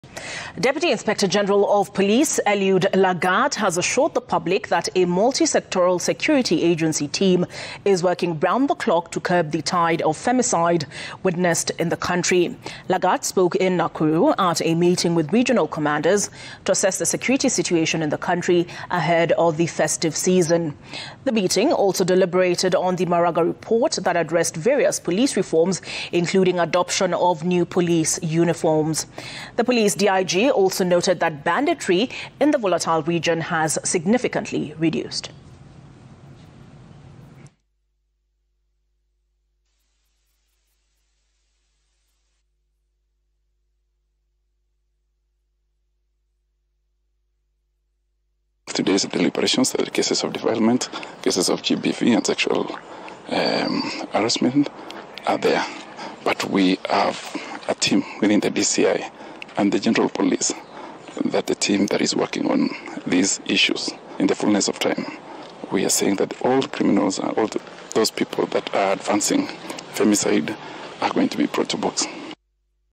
DIG Eliud Lagat assures the public that the police is determined to end rising cases of femicide
Curbing Femicide
Dig Lagat Says Police Determined To End The Vice
Lagat: We’re Working Round The Clock To Curb Femicide
Kenya Has Witnessed A Surge In Murders Targeting Women